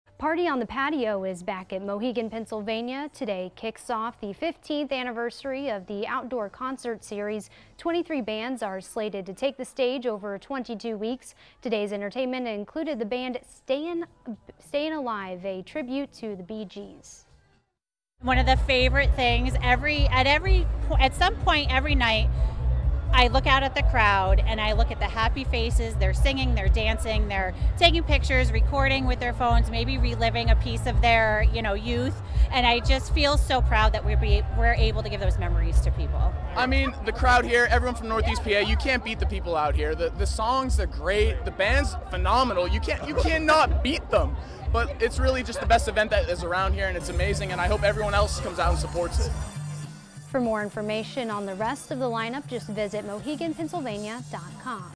'Party on the Patio' returns for its 15th year
Party on the Patio is back at Mohegan Pennsylvania, kicking off the 15th anniversary of the outdoor concert series.

23 bands are slated to take the stage over 22 weeks.